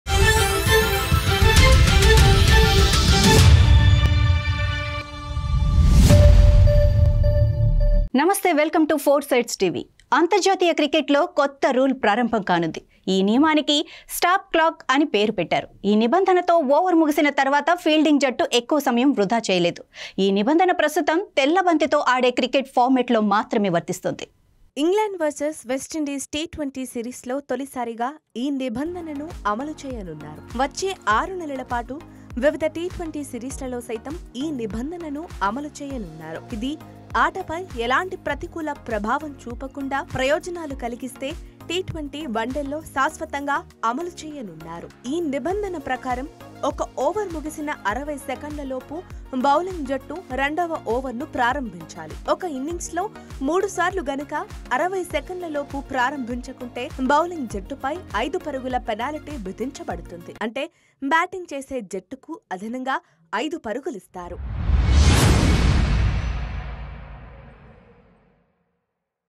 అంతర్జాతీయ క్రికెట్‌లో కొత్త రూల్ || New Rule in International Cricket
New Rule in International Cricket : International Cricket News: This rule will be implemented for the first time in England vs West Indies T20 series. This rule will also be implemented in various T20 series for the next 6 months. It will be implemented permanently in T20 and ODIs without any negative impact on the game and if beneficial.